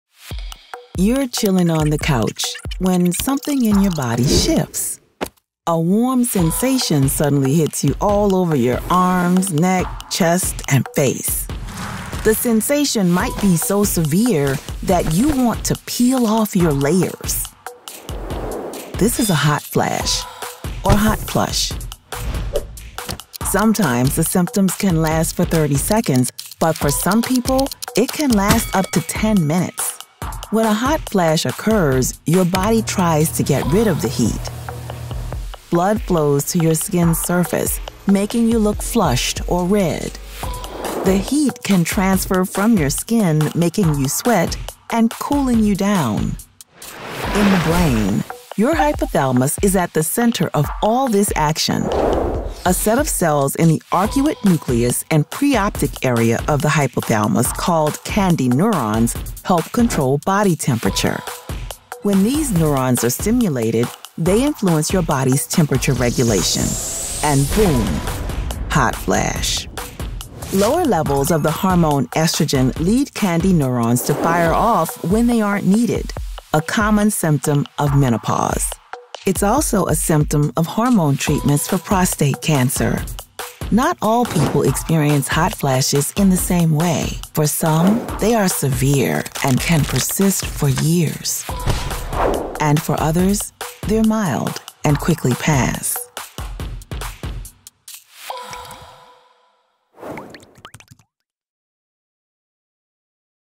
This Is What Happens in Your Brain During a Hot Flash
Hot flashes hit your body with an immediate sense of warmth. They can range from mild to severe, last anywhere from 30 seconds to 10 minutes, and can pop on rare occasions or recur repeatedly for years. This unexpected warm feeling stems from lower levels of estrogen, which fire off neurons in the hypothalamus that control temperature regulation.

Storyboard and animation produced by Midnight Snacks for BrainFacts.org.

Scripting by Chia-Yi Hou.

Editorial Production by Tristan Rivera and Juliet M. Beverly.

Scientific Review by Pauline Maki